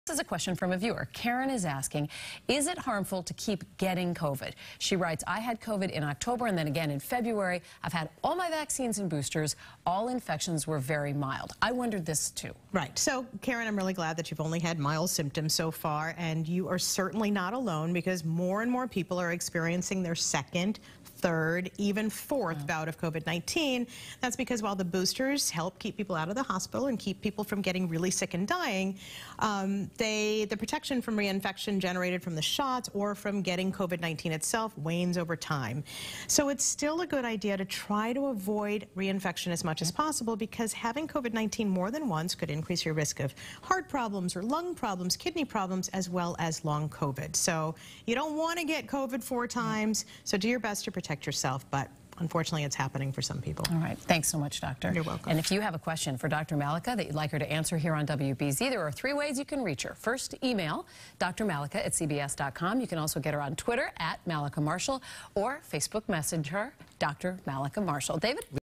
Is COVID reinfection harmful?
Dr. Mallika Marshall answers your questions